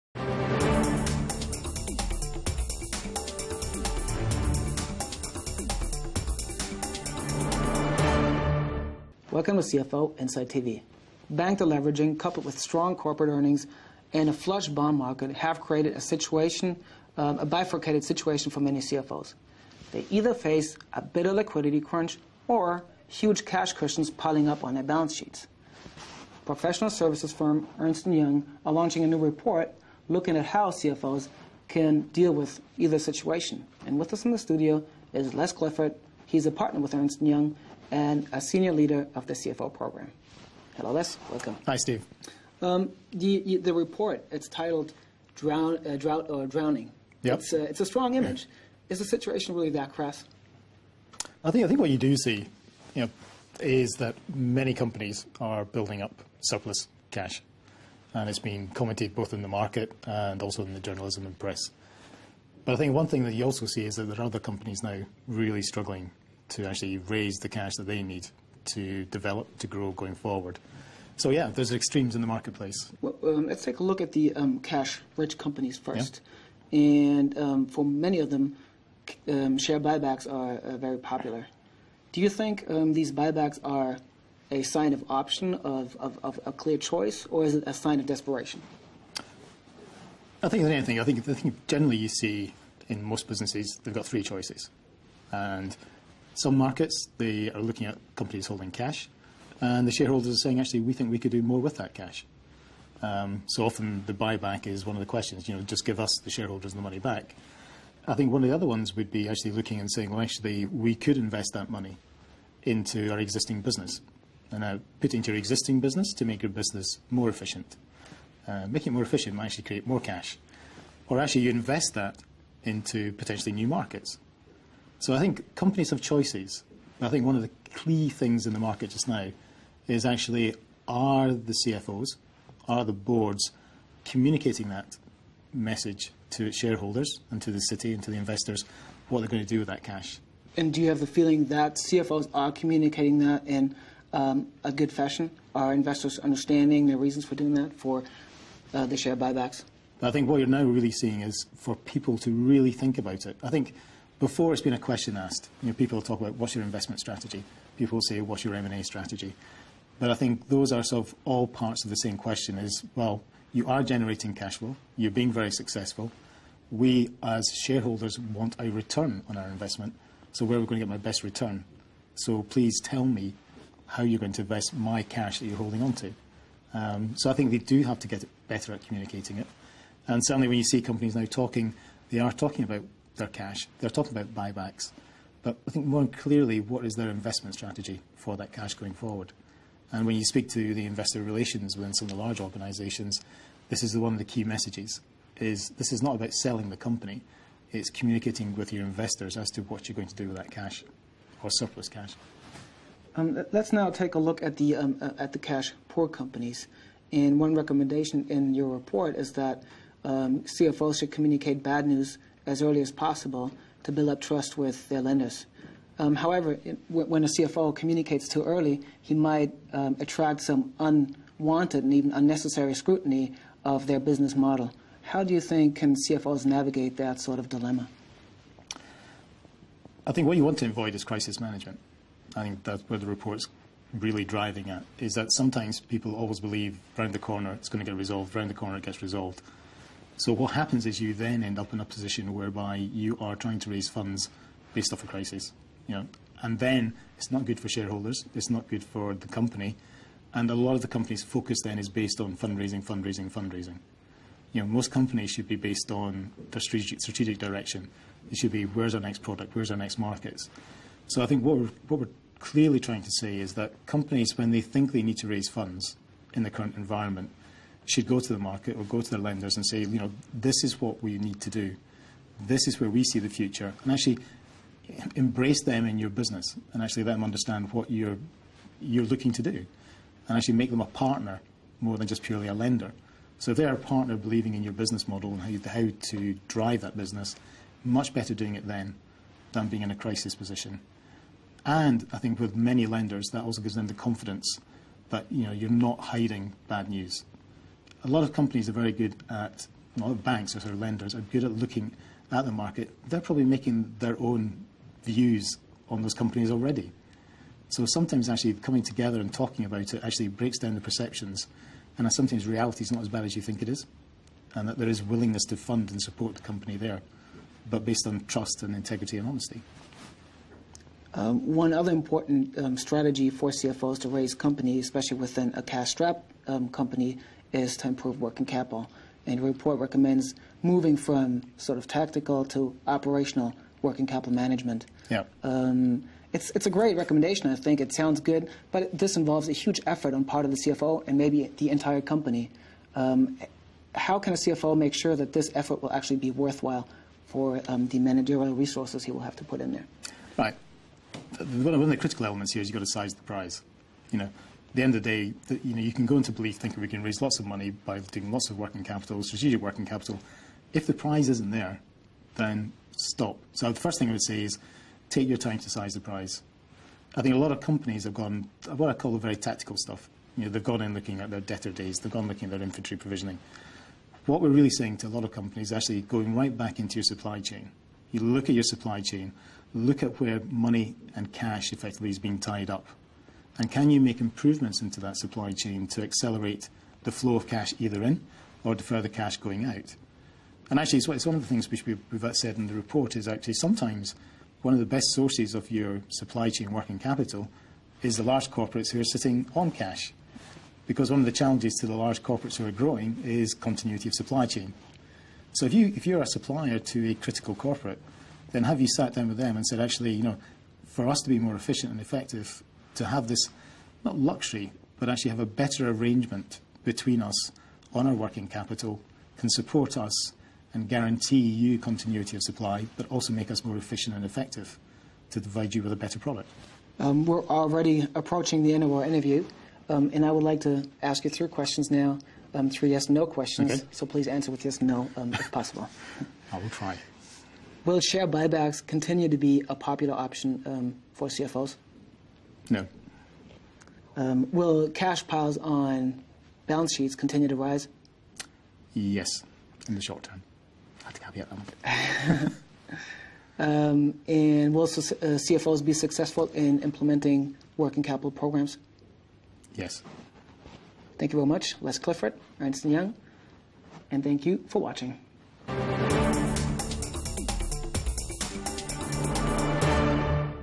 CFO expert Clifford: Communication is key for both cash-rich and cash-poor CFOs
Scarce bank lending coupled with strong corporate earnings and flush bond markets have created a bipolar world for CFOs: they either face a bitter liquidity crunch or huge cash piles building up on their balance sheets. Speaking on CFO Insight TV, Les Clifford, a partner at professional services firm Ernst & Young and a leader of their CFO programme, discusses their new report looking at how CFOs on both sides of the divide can cope with the challenge.
For chief financial officers at cash-strapped companies, raising liquidity is a daily challenge. This requires open communication with the lenders, Clifford says. “What you want to avoid is crisis management. Oftentimes many people try to believe that around the corner it’s going to be resolved, there’s going to be cash.” Instead of open discussions about long-term solutions with their lenders, they opt for short-termism.
However, this can be a dangerous approach, Clifford warns. “Most lenders are good at looking at the market and making their own views of these companies already,” he says. “So coming together to discuss each other’s perception can break down walls. ... Make [the lenders] a partner more than purely a lender.
Chief financial officers at cash-rich companies seem to face the easier task. However, they too must ensure that their message is being understood, Clifford says. “The question is: Are CFOs communicating their decision clearly enough to investors?” Many finance chiefs may not do all they can in this respect, leaving them vulnerable to investor backlash.